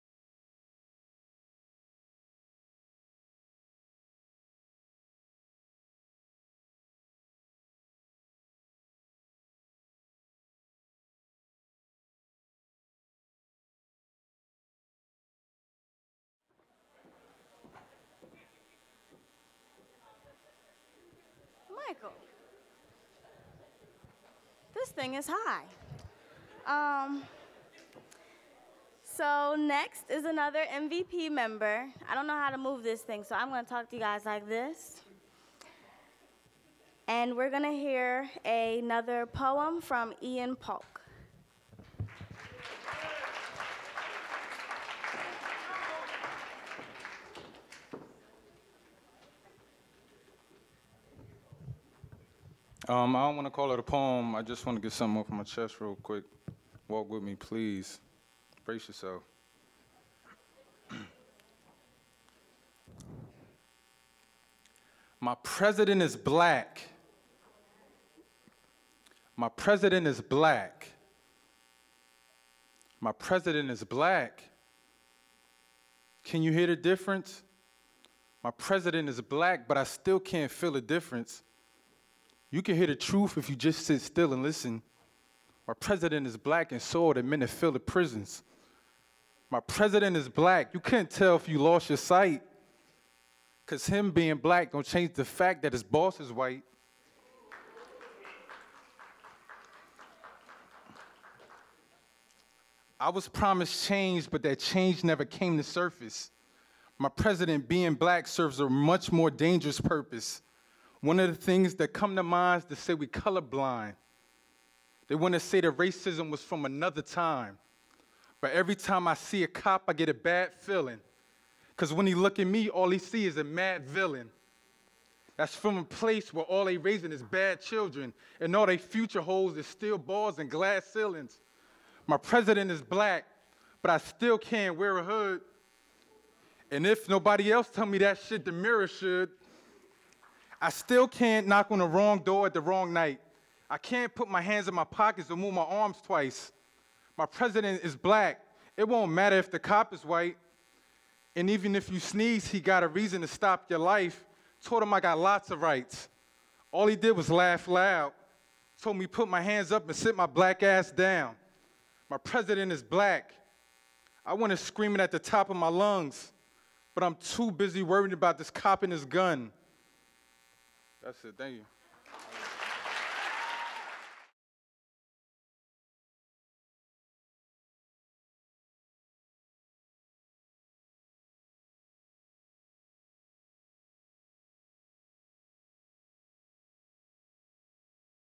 The Night Of Truth - Ean Polke [HD]
On December 2nd 2013, The RUMVPSO hosted an event called "The Night Of Truth". The Night Of Truth provided a platform for artists who wanted to show case their talents but never felt comfortable performing, who wanted to tell people how they really feel about social issues but could never find the courage, and who just wanted to express themselves in a way that they were afraid their friends wouldn't understand. At "The Night Of Truth" the performers put aside all of their fears and discussed the issues that matter through art!